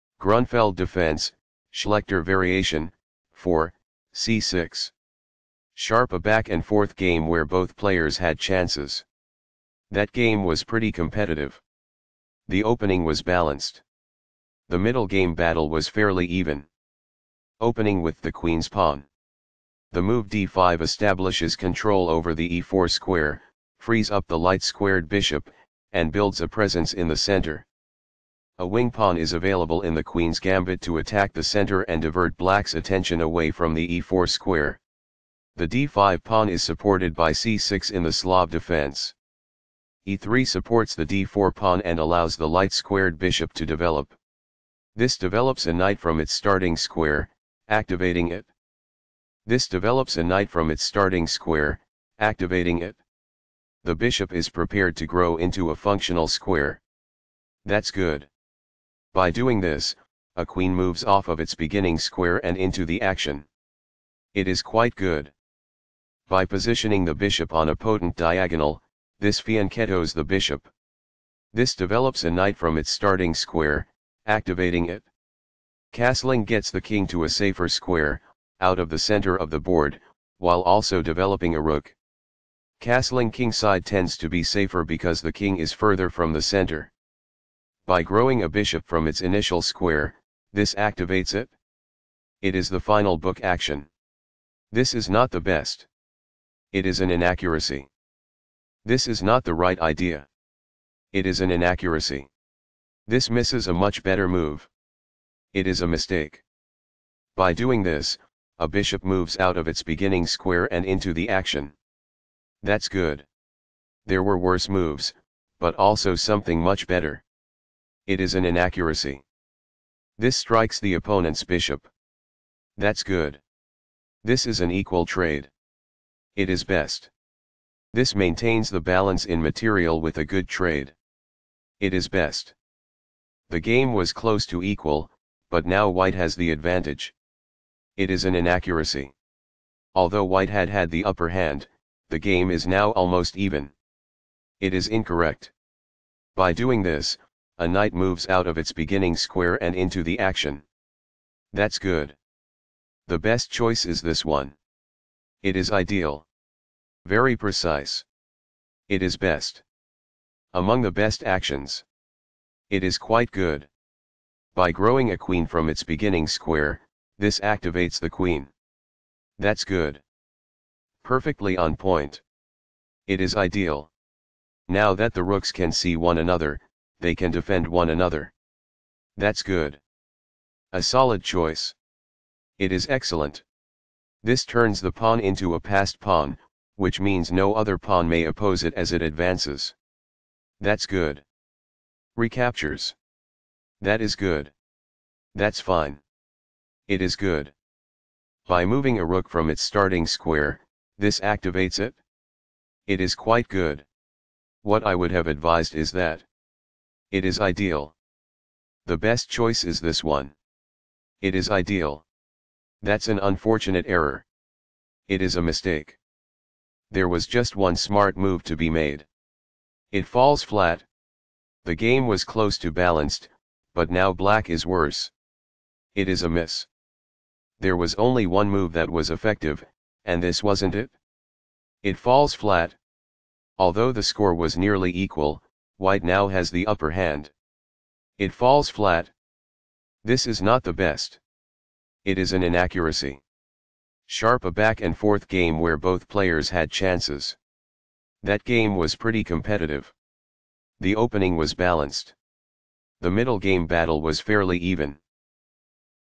White Rakhmanov, Ale, Black Batchuluun, T.,Slav Defense: 3.e3, Event 26th Universiade Men, Site She
Slav Defense: 3.e3, Event 26th Universiade Men, Site Shenzhen CHN, Date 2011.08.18, Round 5, White Rakhmanov, Ale, Black Batchuluun, T., Result 1-0, ECO D10, WhiteElo 2585, BlackElo 2427, PlyCount 43, EventDate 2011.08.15, EventRounds 9, EventCountry CHN, Source Mark Crowther, SourceDate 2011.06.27, WhiteTeam RUS, BlackTeam MGL